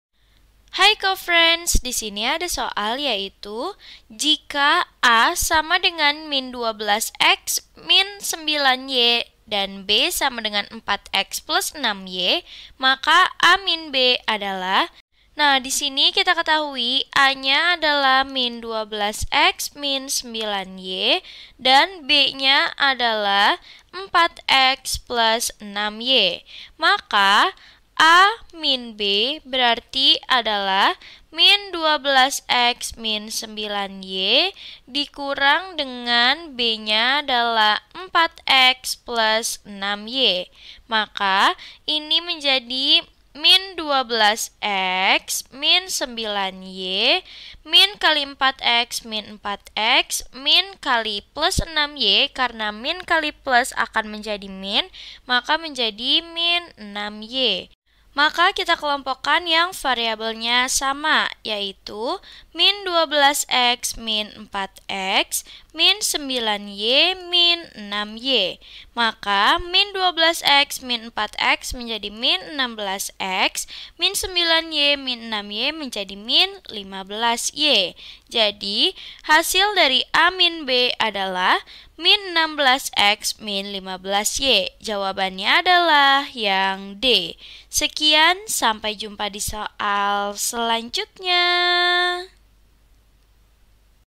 Jika A=-12x 9y dan B = 4x + 6y maka = A - B adalah 3v ~8x 3y ~l6x a. C. 15y 15y ~8x d. ~l6x b. S 5
Topik atau Materi: Operasi Hitung pada Bentuk Aljabar - OPERASI DAN FAKTORISASI BENTUK ALJABAR - ALJABAR - Matematika Kelas 7 SMP - KTSP

Sumber Soal: Matematika Kelas 7, KTSP, JD, 2011 , Depdiknas

00:00 : Bedah Soal
01:00 : Konsep, rumus dan pengertian Operasi Hitung pada Bentuk Aljabar
01:18 : Langkah penyelesaian soal
00:34 : Jawaban akhir
01:04 : Penutup